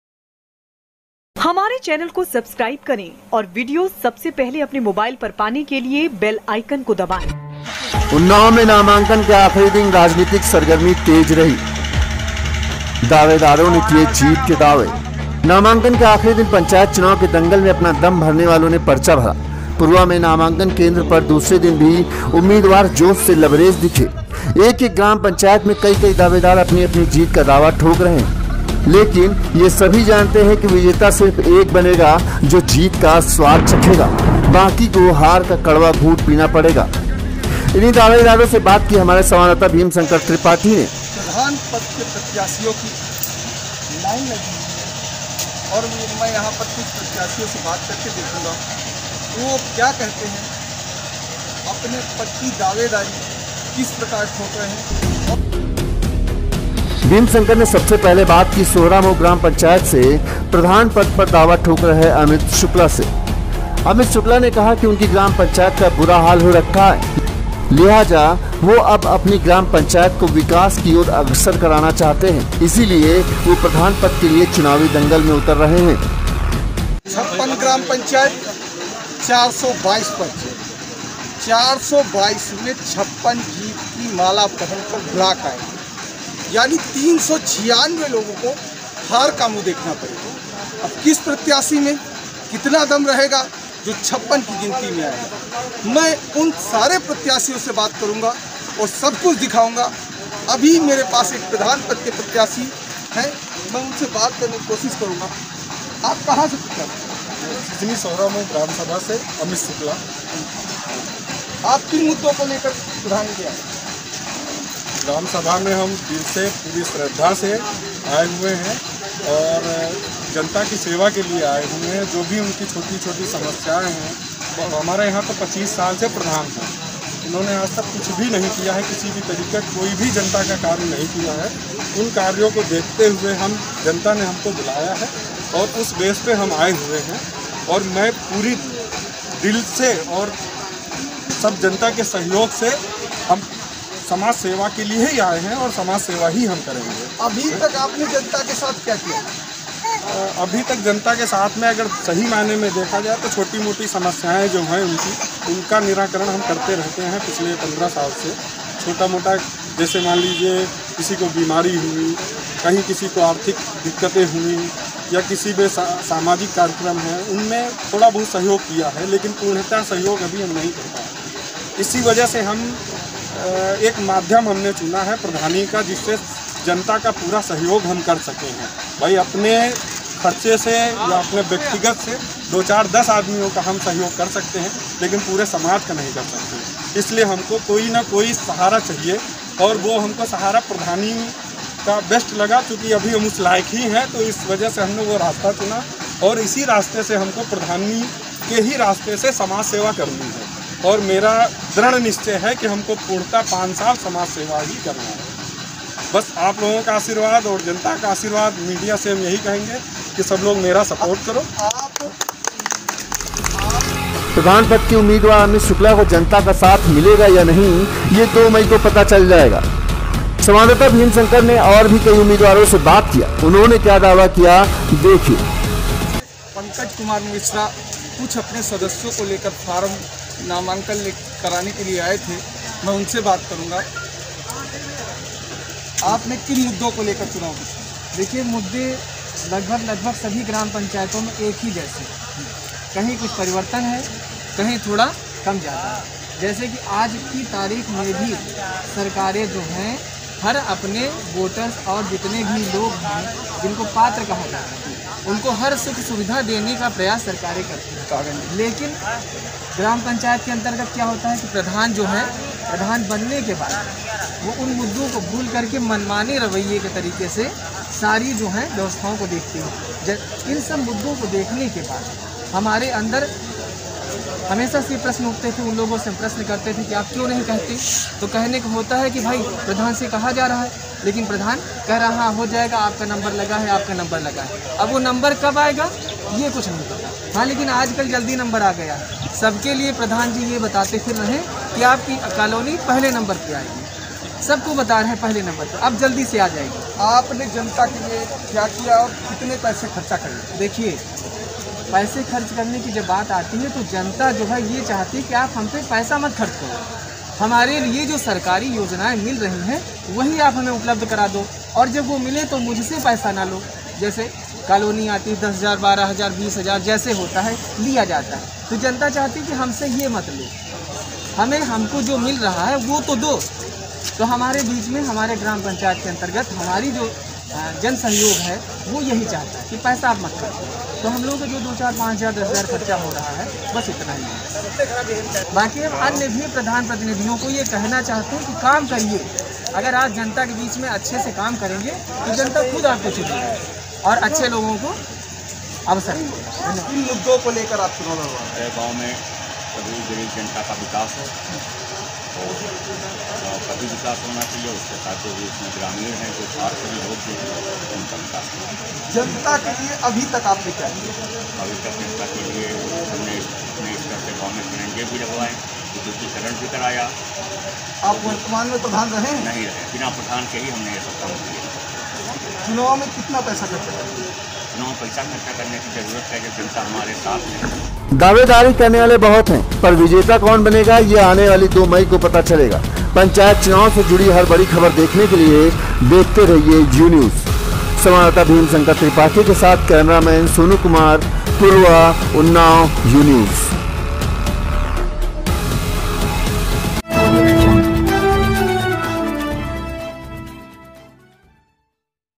Unnao Gram Panchayat उन्नाव में प्रधान पद के दावेदाराें ने क्या कहा देखिए
उन्नाव में ग्राम पंचायत चुनाव का नामांकन पूरा हो चुका है, अब दावेदार अपने अपने दावे जनता के सामने रख रहे हैं